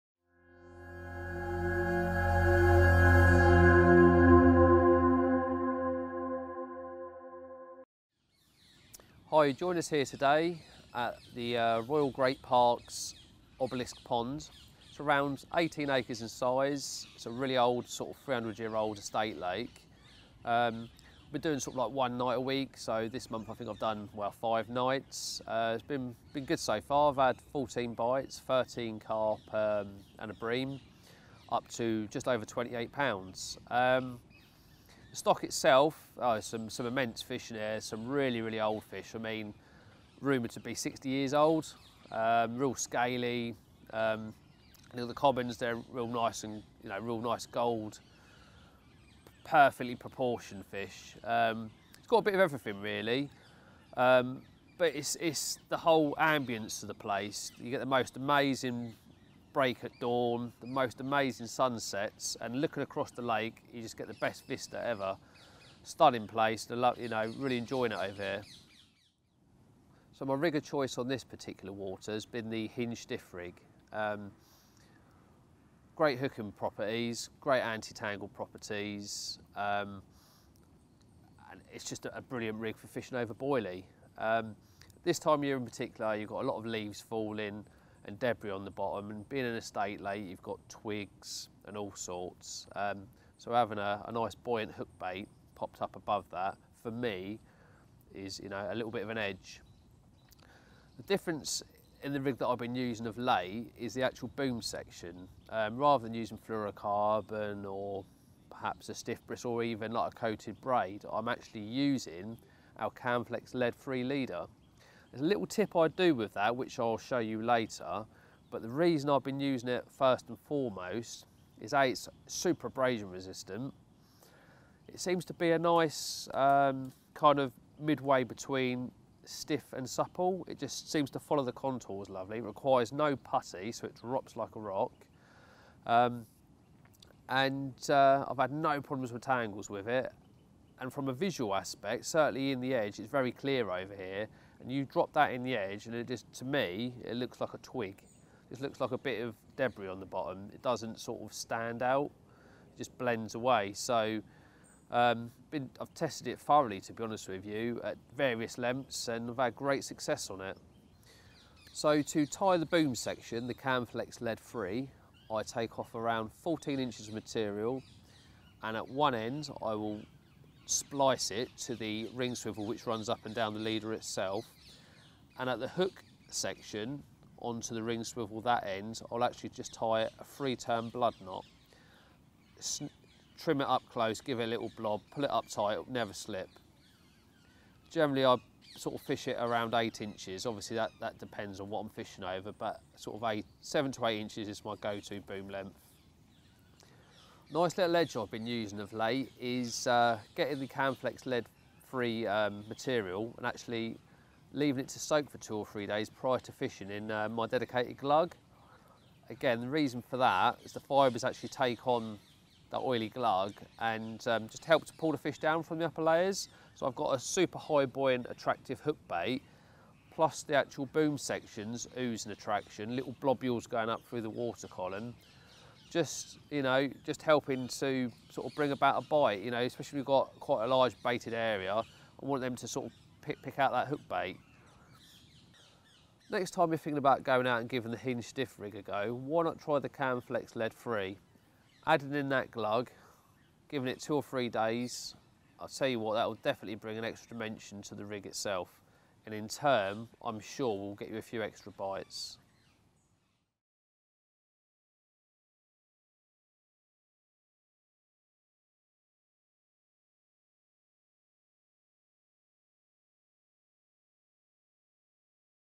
Carp Fishing Hinged Stiff Rig Boom Section With A Difference
We join Gardner Tackle team member Ian Lewis at the Royal Great Park's Obelisk Pond, where he reveals a slightly different boom section he has been using to great effect with the infamous Hinged Stiff Rig.